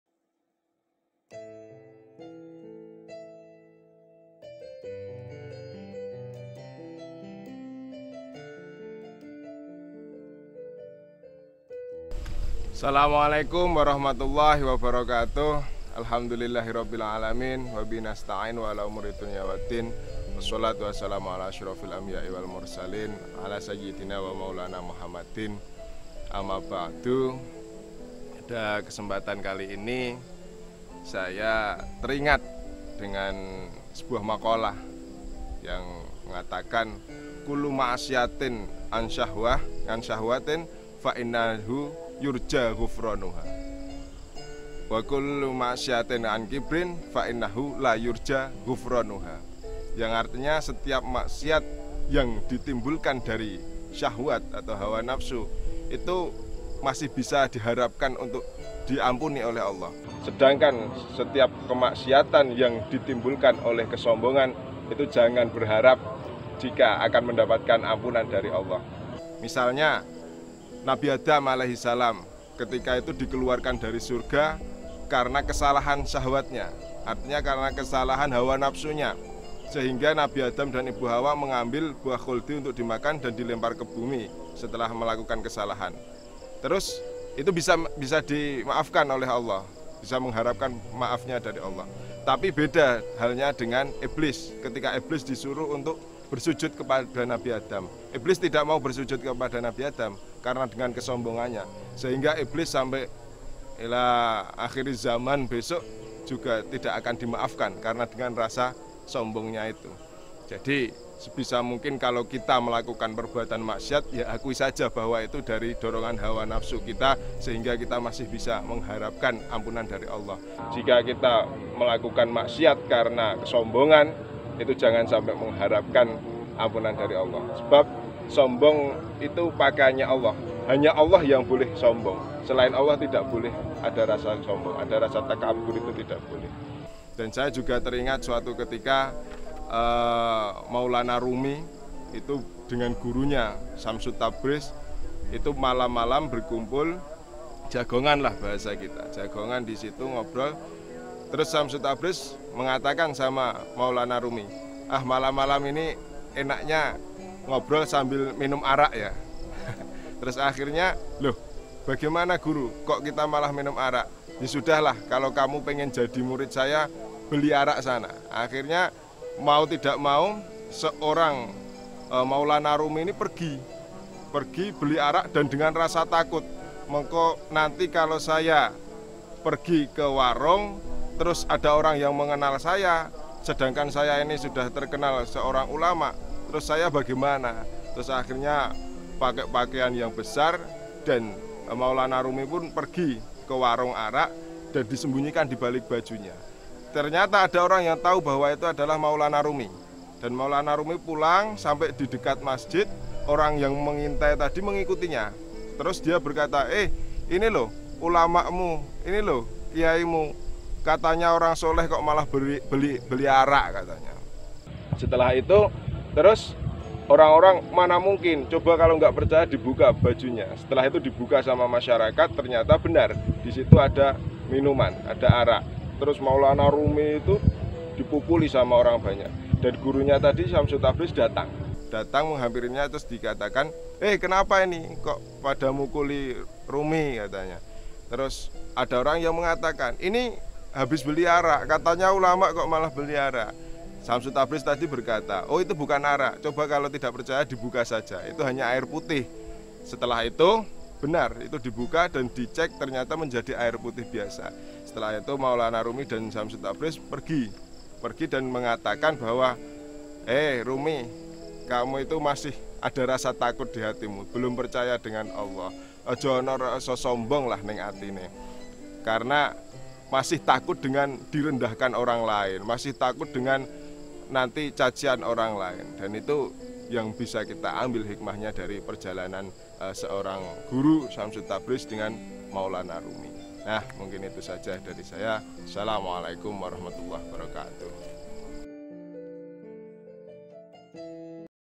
Ngaji Romadhon 8_Ahmad Muhammad Naufal El-Busthommy: Takabur I Ansor Banser Pleret
Ngaji Romadhon #8 Takabur

Setiap maksiat yang ditimbulkan dari syahwat atau hawa nafsu itu masih bisa diharapkan ampunan dari Allah SWT. Sedangkan setiap kemaksiatan yang ditimbulkan oleh kesombongan  jangan berharap jika akan mendapatkan ampunan dari Allah SWT. Misalnya Nabi Adam as, ketika itu dikeluarkan dari surga karena kesalahan syahwatnya, karena kesalahan hawa nafsunya sehingga Nabi Adam dan Siti hawa mengambil buah Khuldi untuk dimakan dan diturunkan ke bumi setelah melakukan kesalahan...